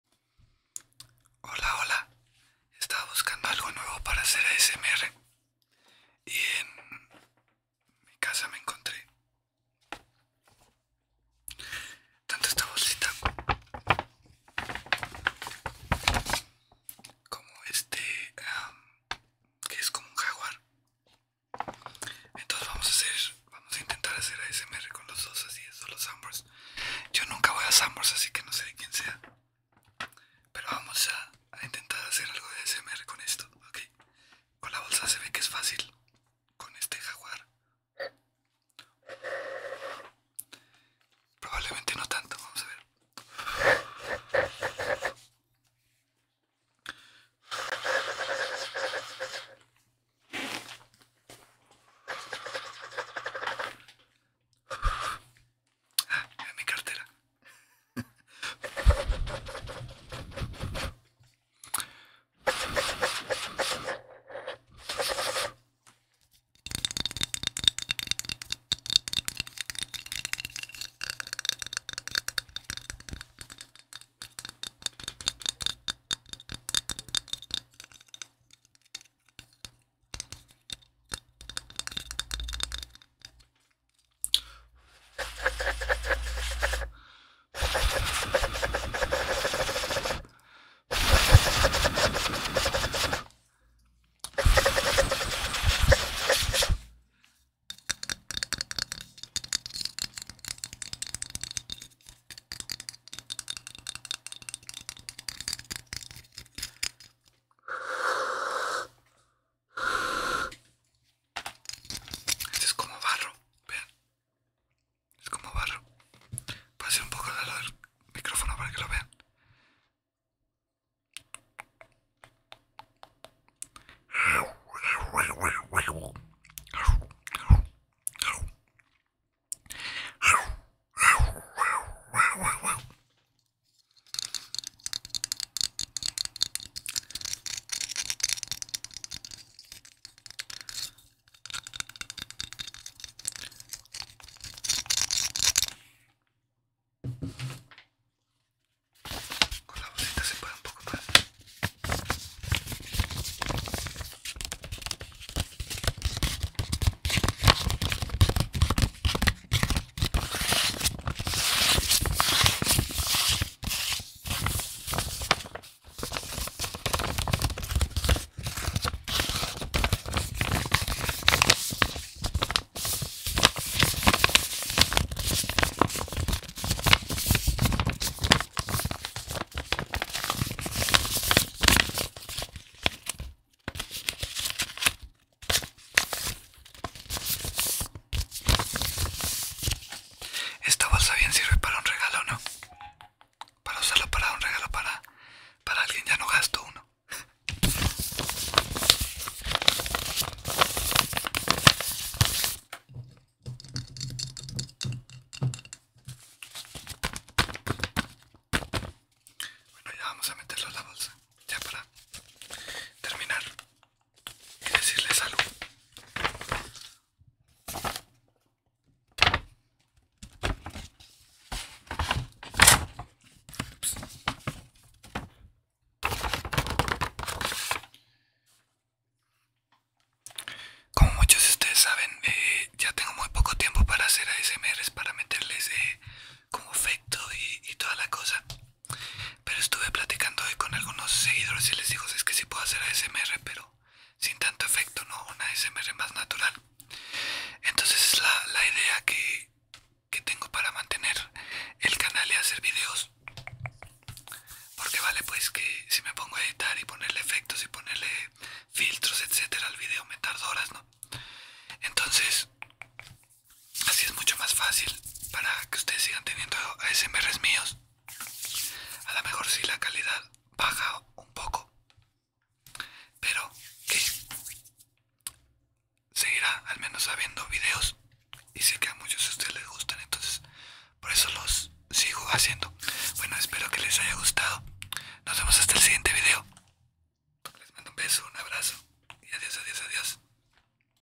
ASMR solo Sanborns para personas Consumistas. 💸
Hoy venimos con el ASMR de solo Sanbrons para todos aquellos que les guste el consumismo.

Mi canal de espectaculos
Mi canal de noticias